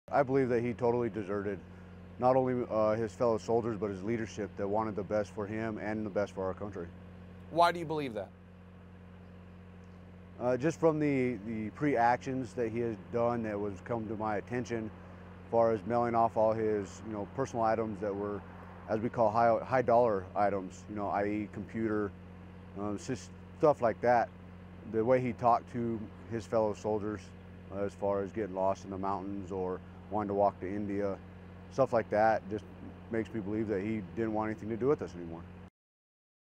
BERGDAHL'S FMR SQUAD LEADER-DESERTED
Bergdahl's Former Squad Leader: I believe that he totally deserted not only his fellow soldiers but his leadership.